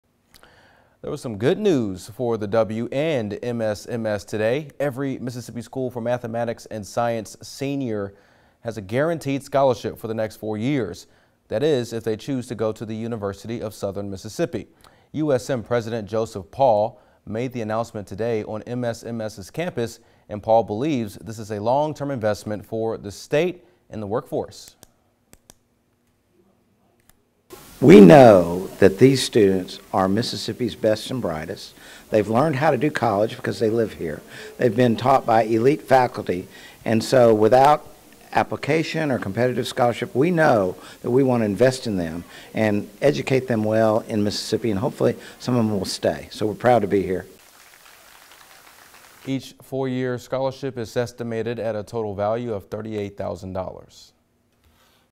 MSMS seniors guaranteed full scholarships at Southern Miss
Every Mississippi School of Mathematics and Science senior is guaranteed a four-year scholarship if they attend the University of Southern Mississippi.
Each four-year scholarship is estimated at a total value of $38,000.